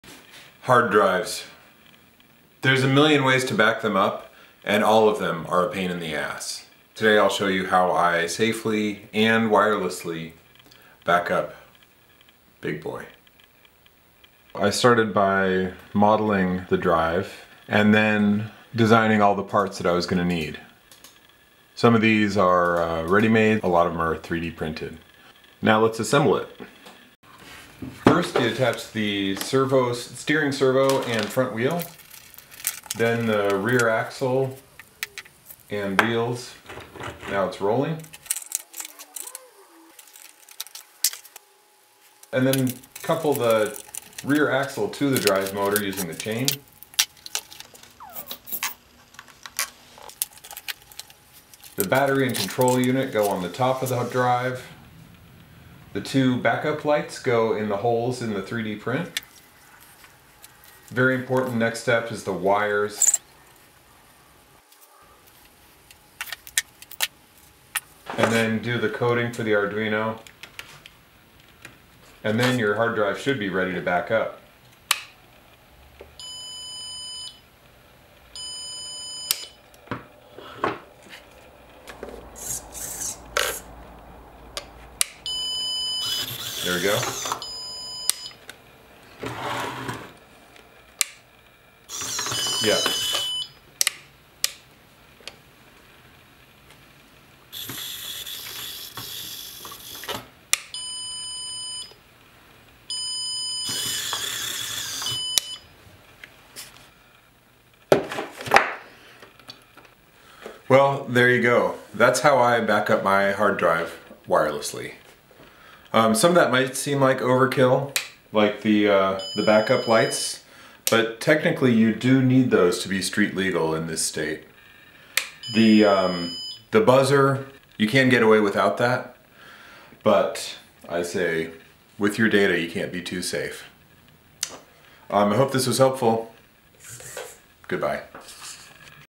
Hard Drive Backup
Let me show you the method I used to safely, wirelessly, and remotely backup my hard drive.